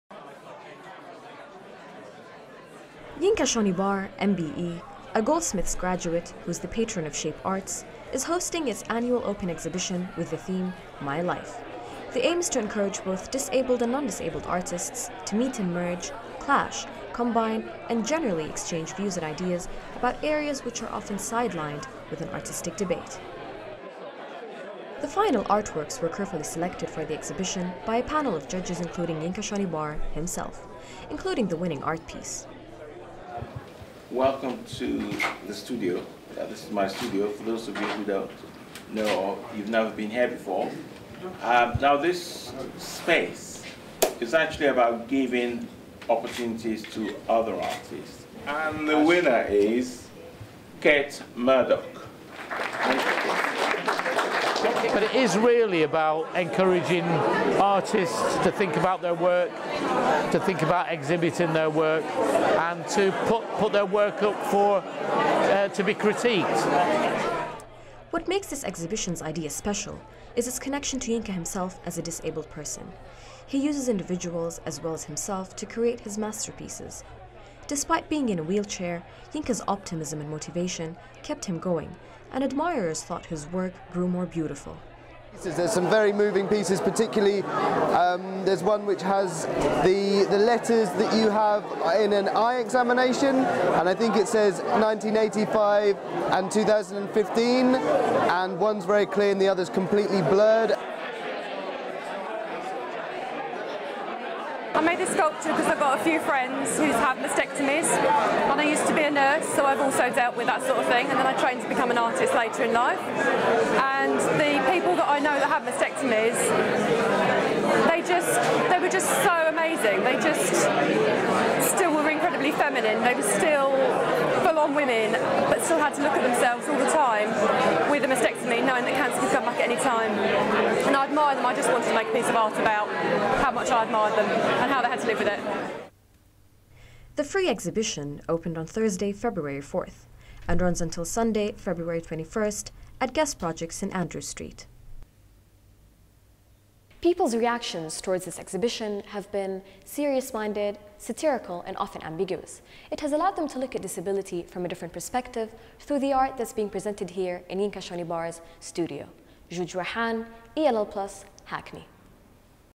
Yinka Shonibare Hosts Shape Open Exhibition
An exhibition encouraging artists to express life through the lens of disability was on show at Yinka Shonibare’s Guest Projects Space in Hackney. Jude Jweihan reports.